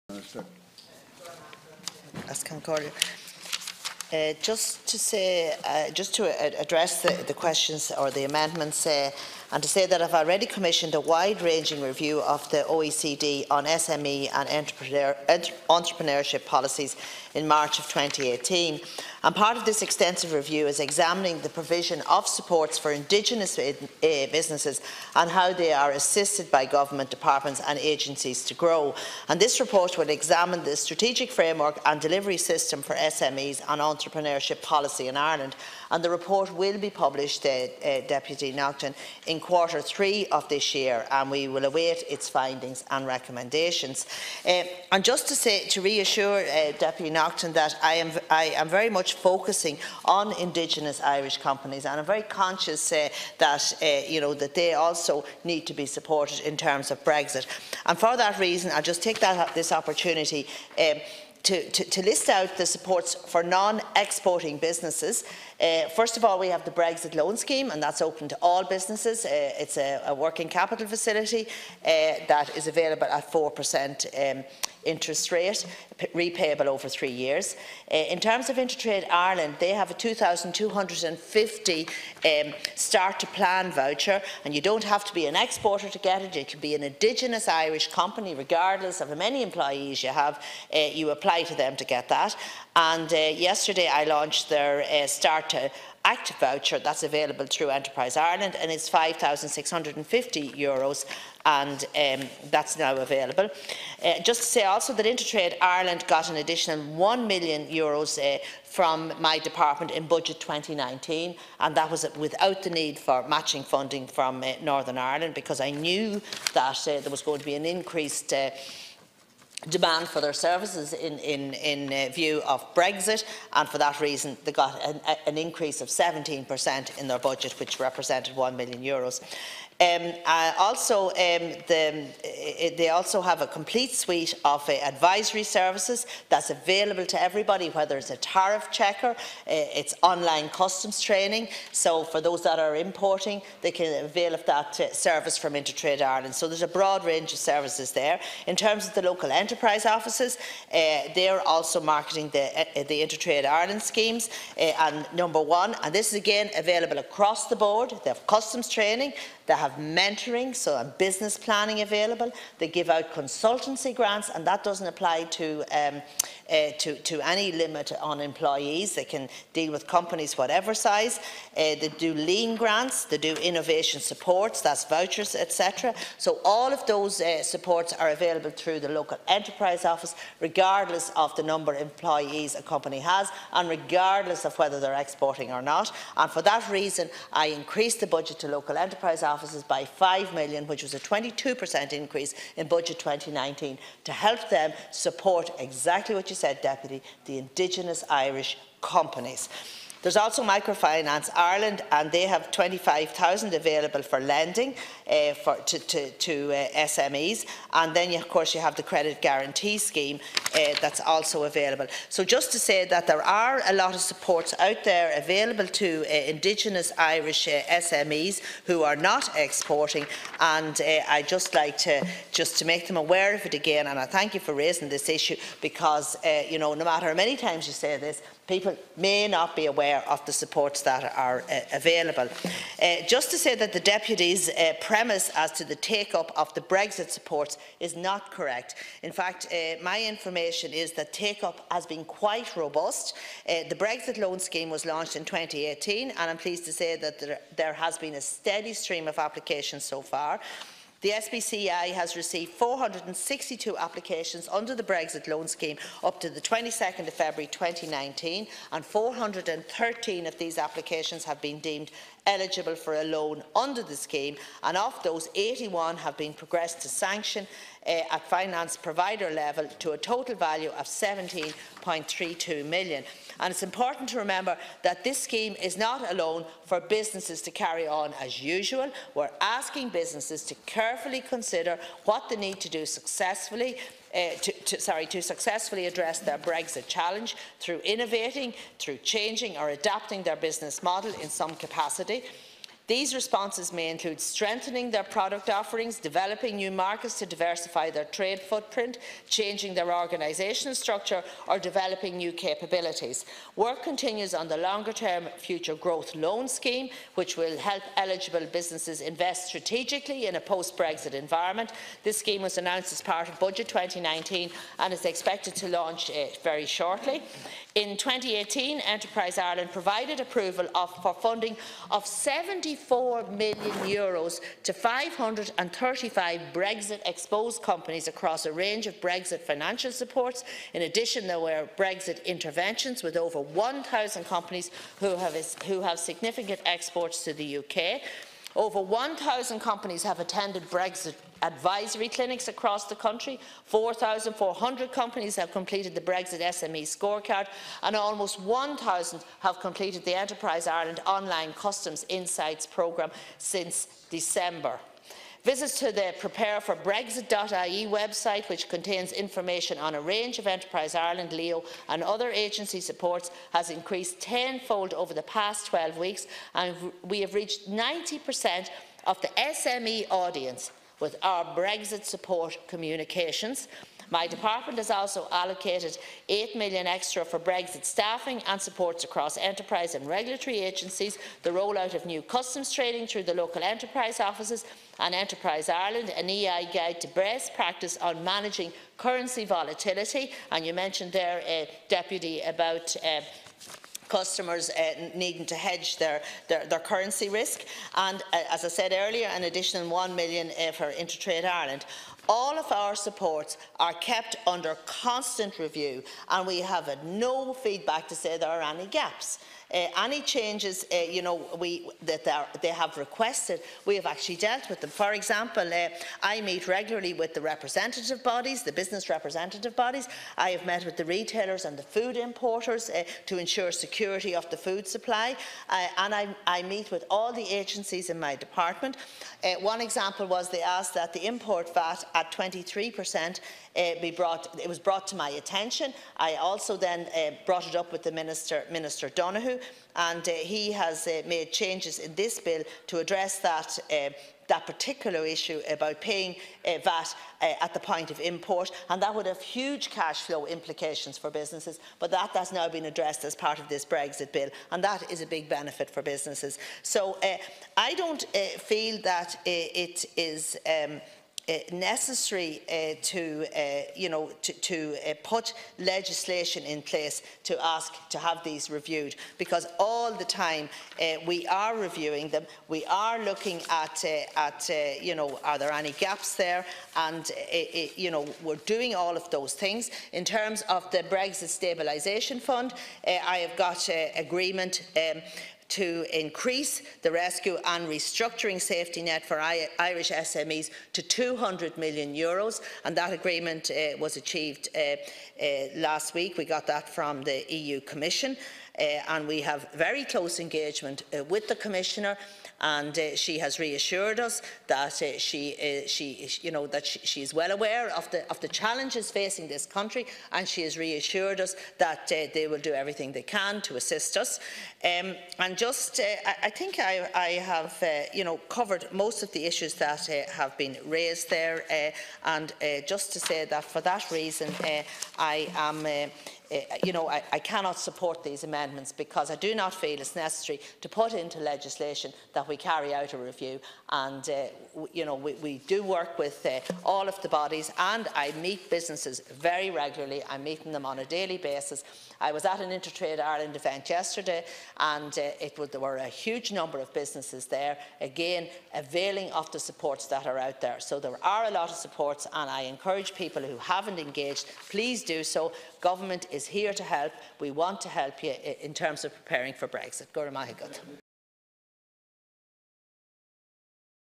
Deputy Heather Humphreys - Government Business - 05.03.2019
Dáil Éireann debate - 5 Mar 2019

Withdrawal of the United Kingdom from the European Union (Consequential Provisions) Bill 2019 - Committee and Remaining Stages